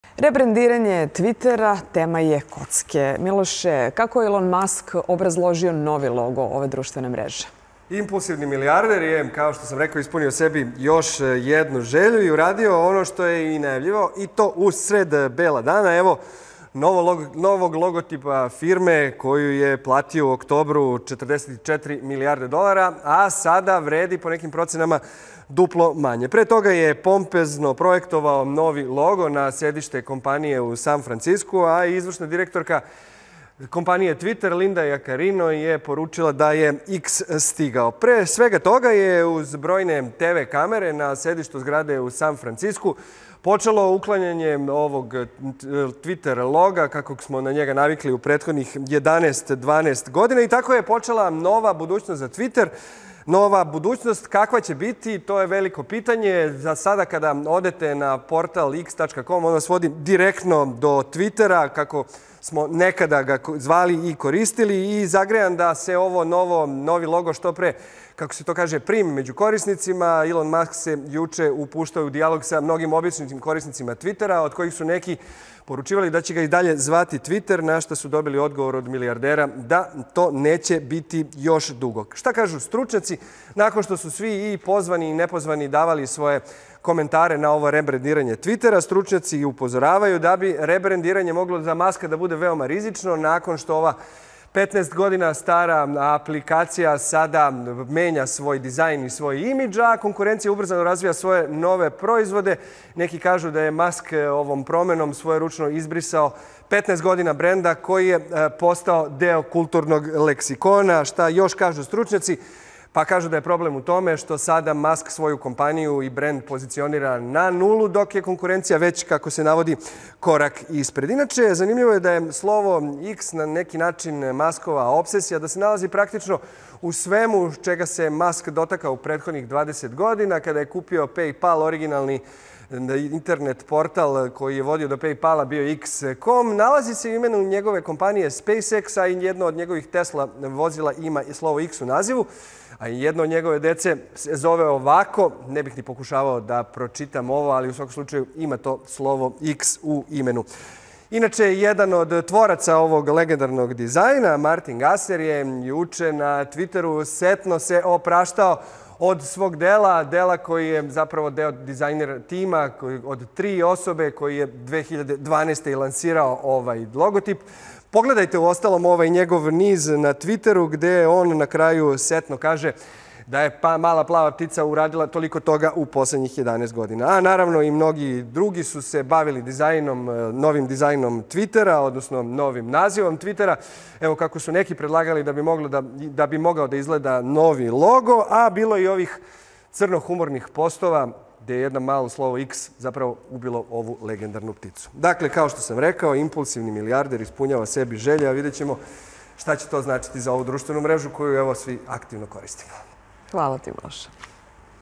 Znak X umesto pepoznatljive plave ptice - koliko će to da košta ekscentričnog milijardera?
Ilon Mask je, kako je i ranije najavljivao, promenio prepoznatljivi logo Tvitera, a plavu ptičicu zamenilo je slovo X. To je otvorilo pitanje kako će ova promena uticati na ovu društvenu mrežu.